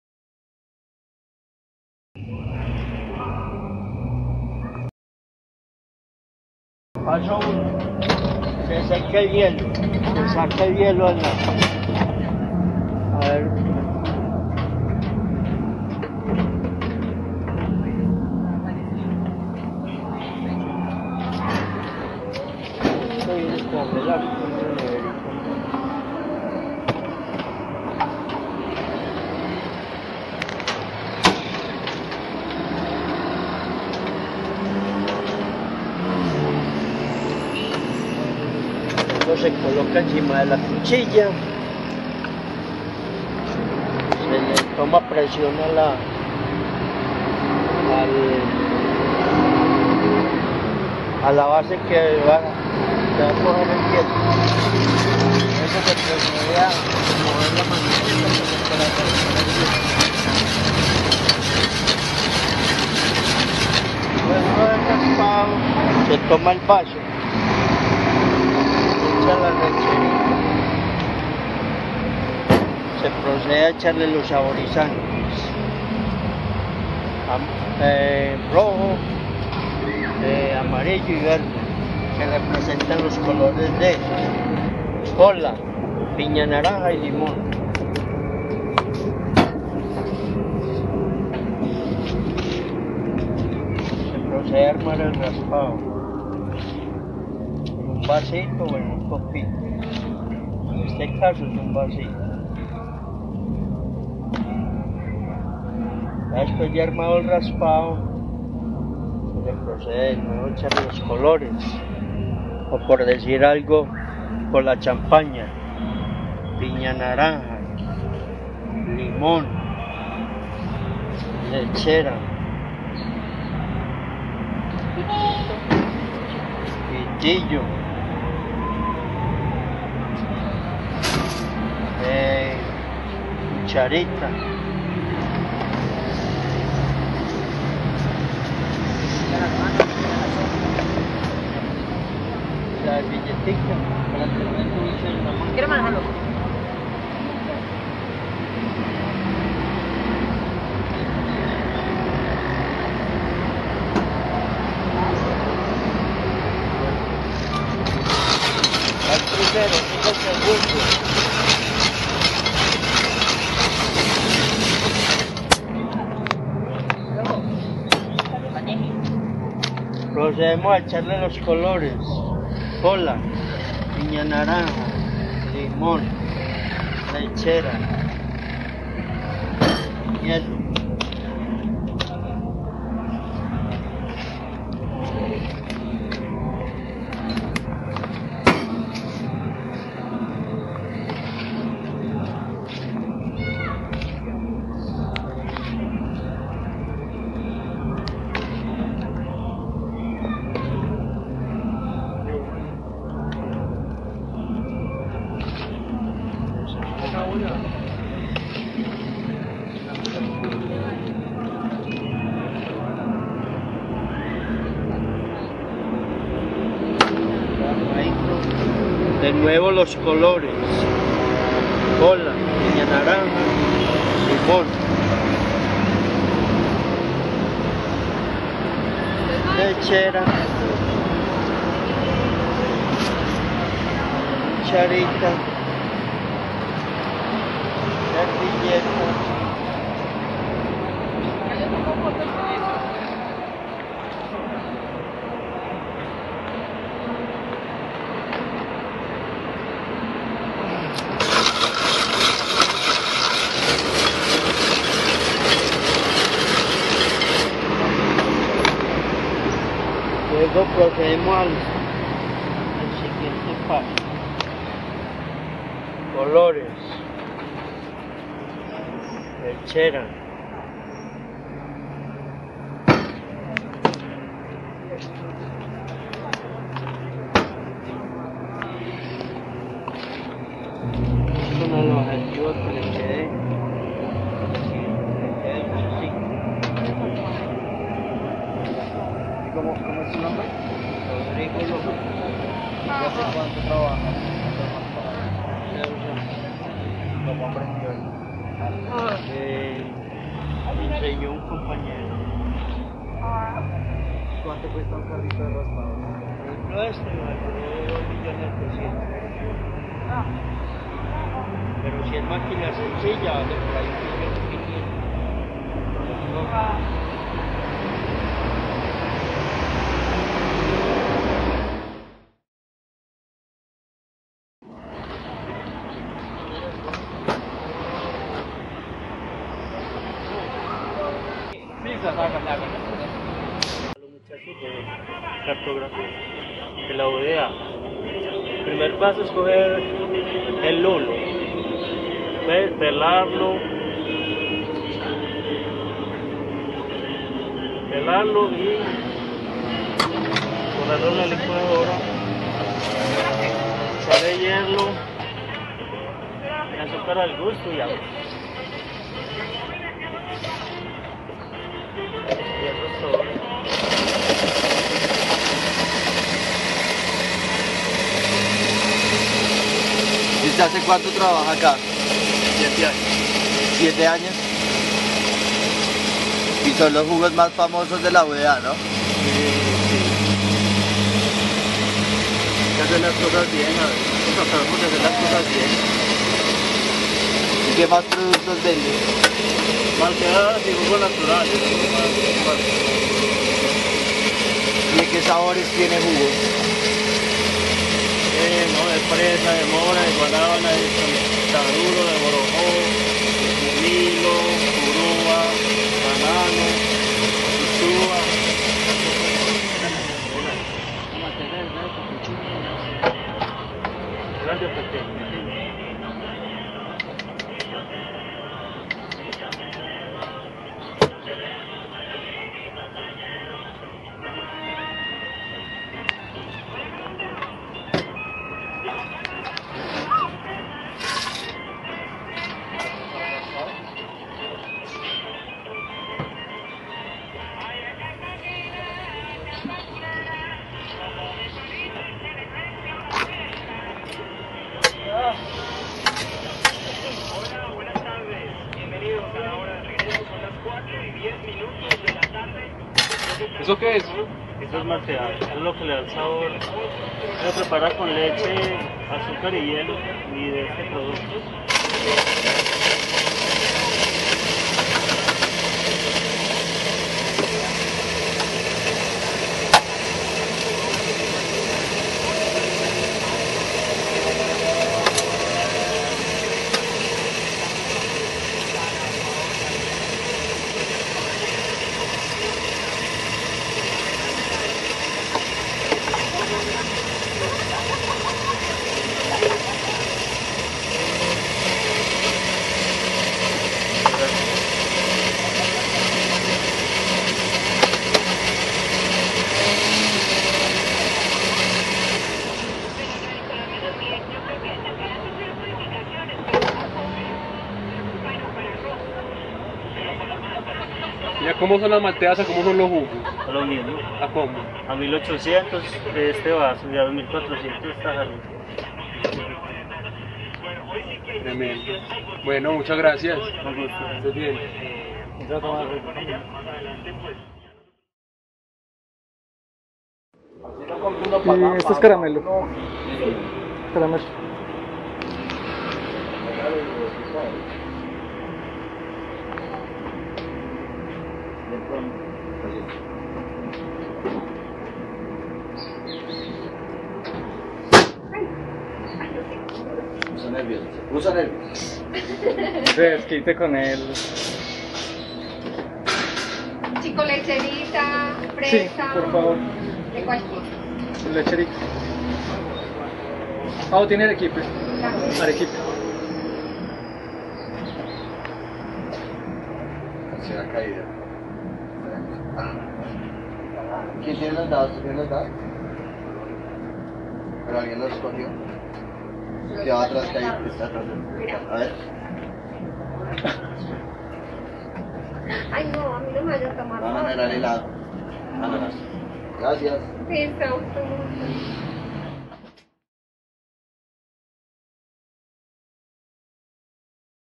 Taller Cartografías del Azar Medellín 2
Taller de Cartografías del Azar realizado por Camilo Cantor en el MDE11
recorridos del segundo día